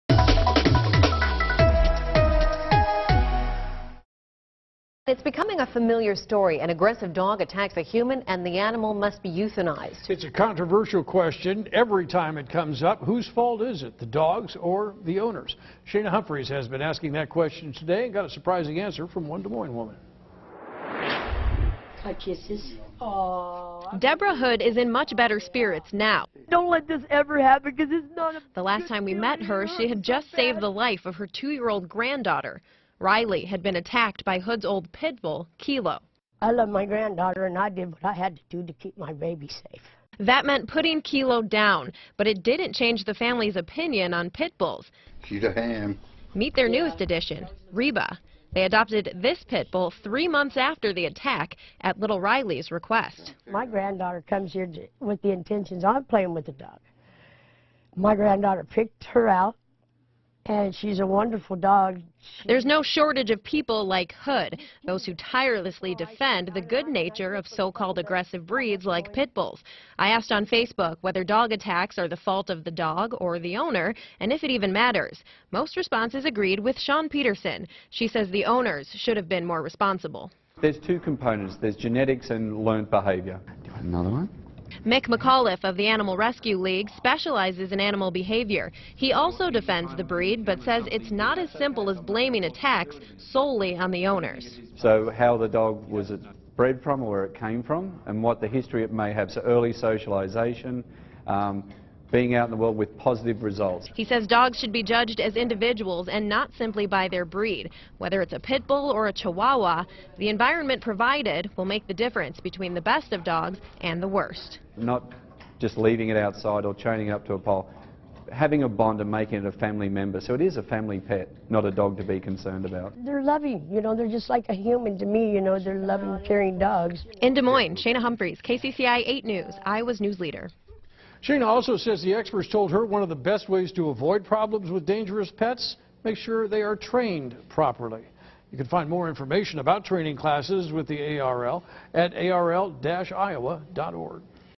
Family adopts new pit bull months after attack
The debate continues over pit bulls after a West Des Moines City Council meeting.